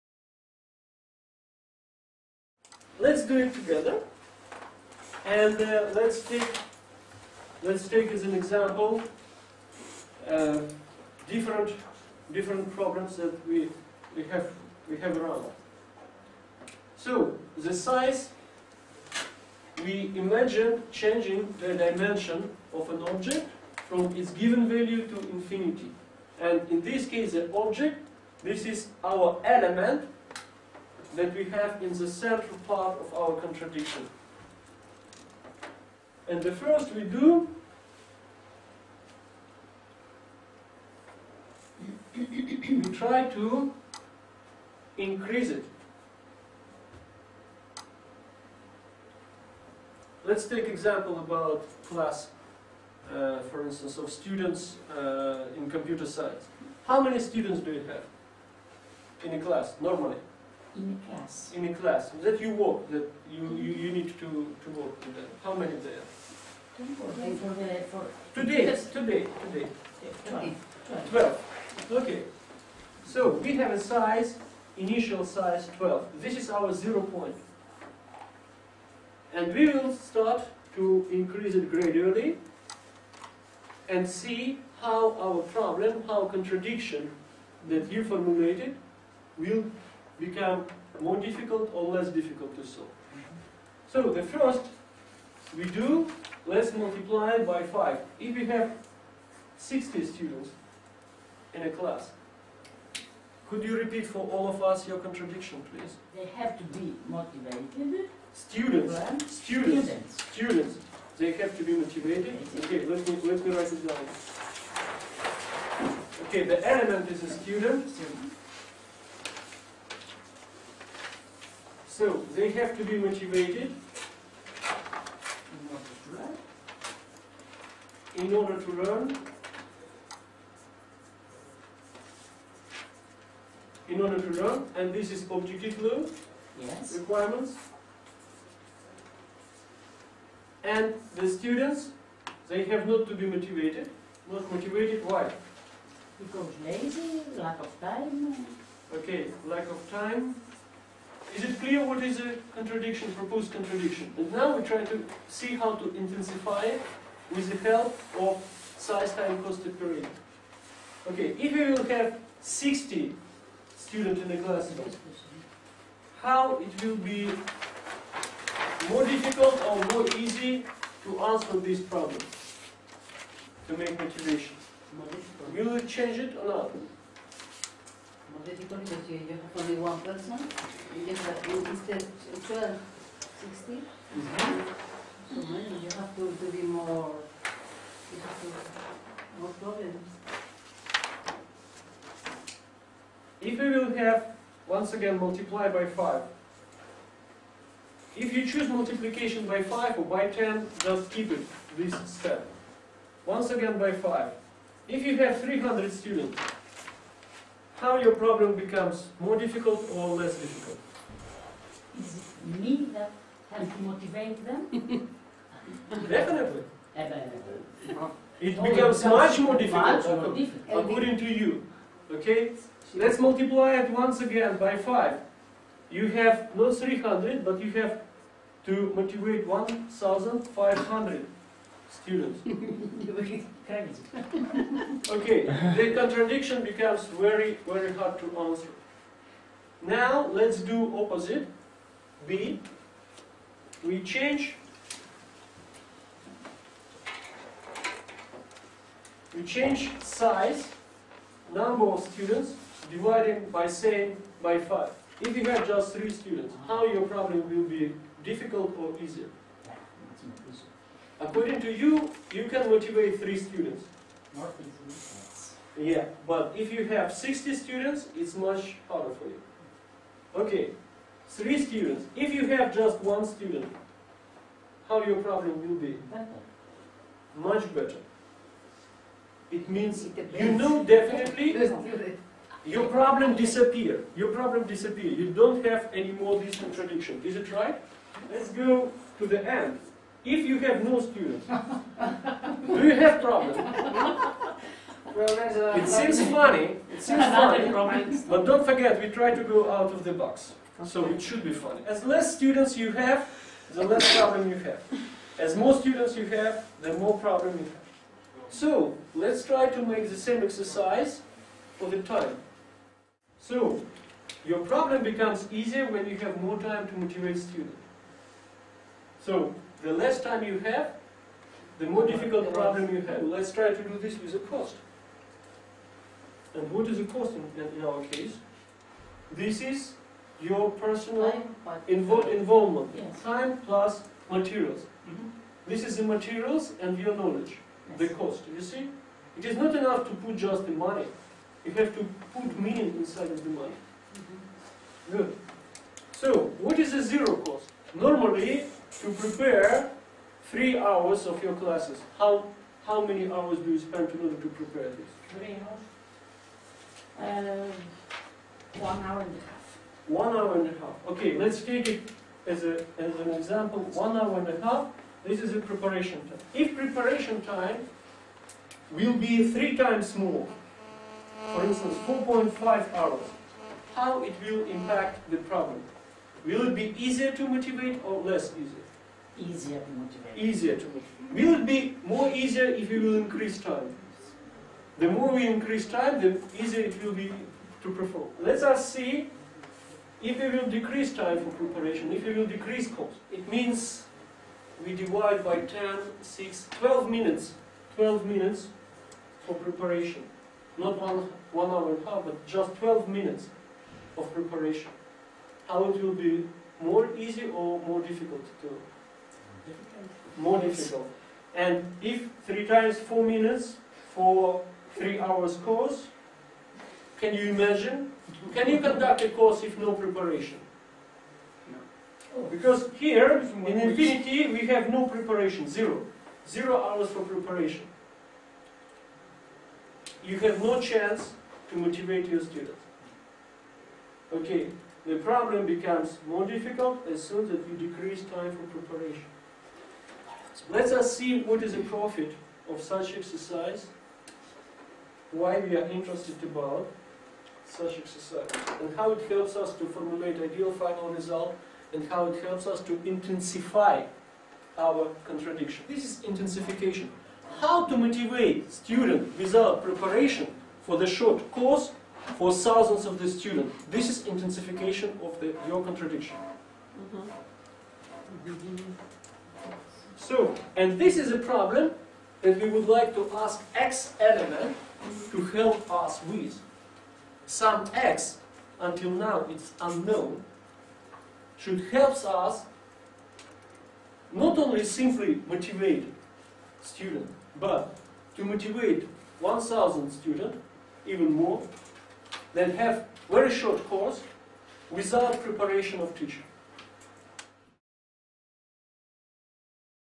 Course | TRIZ | Size-Time-Cost Operator: Practical Application
(23 Feb 2010) Dmitry Kucharavy shows how the application of the Size-Time-Cost Operator helps one intensify the contradiction faced in the analysis of the problem.

Find out more about the possibility to get a grant for attending the course "Problem Management and Effective Education Based on TRIZ"